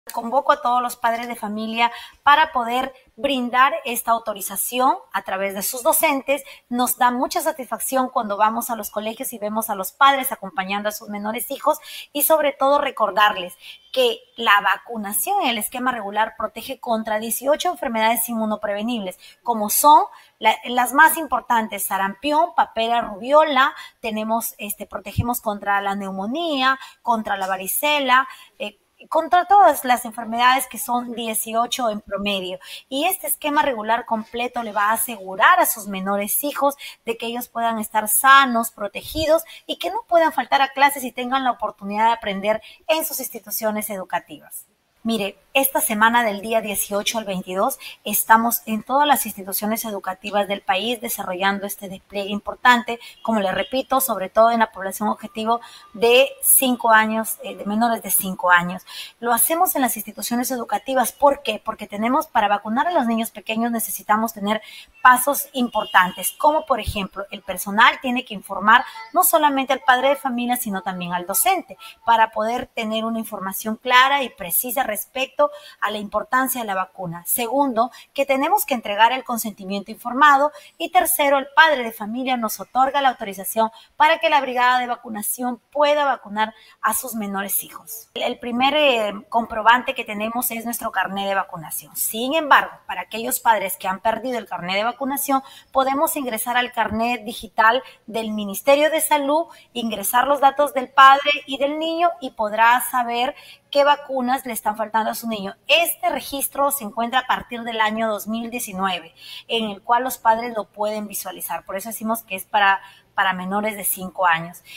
Esquema de vacunación completo protege contra 18 enfermedades a tu niño
El Minsa viene desarrollando una jornada de vacunación en colegios para que los niños menores de 5 años puedan completar su esquema de vacunación. Conversamos con Magdalena Quepuy, Directora de Inmunizaciones del Minsa.